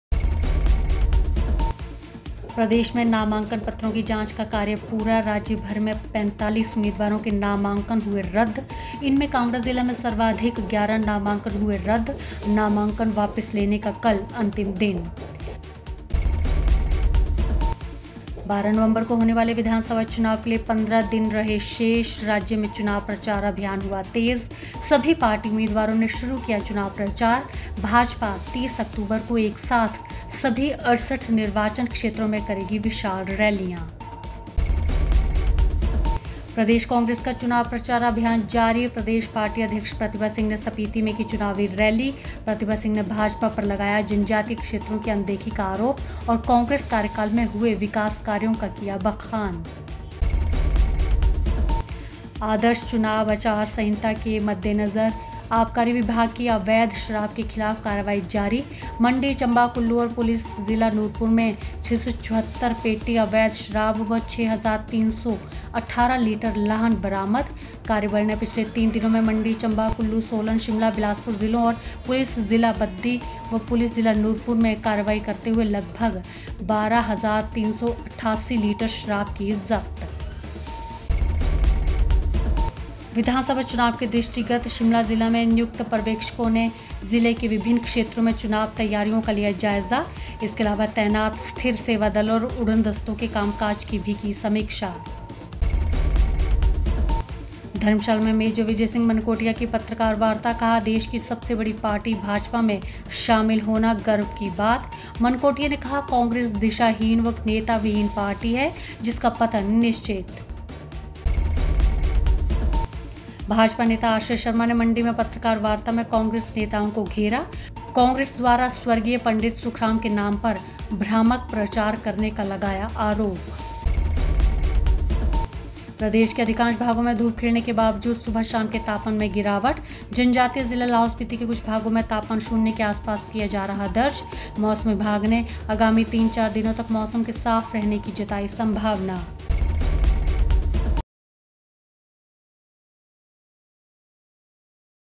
हिमाचल फटाफट खबरें
प्रदेश में 45 उम्मीदवारों के नामांकन हुए रद्द-देखें हिमाचल फटाफट खबरें